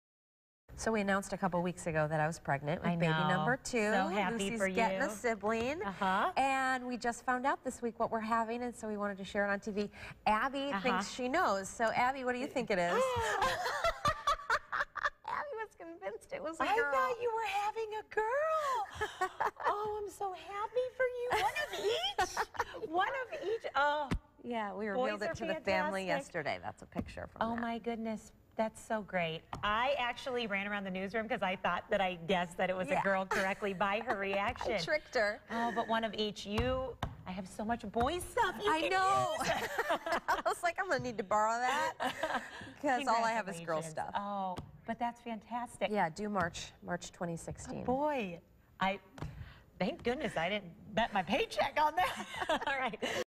IT'S A BOY! Kristin Bien announces new baby's gender!
WSBT 22's Kristin Bien is having a baby boy! _________________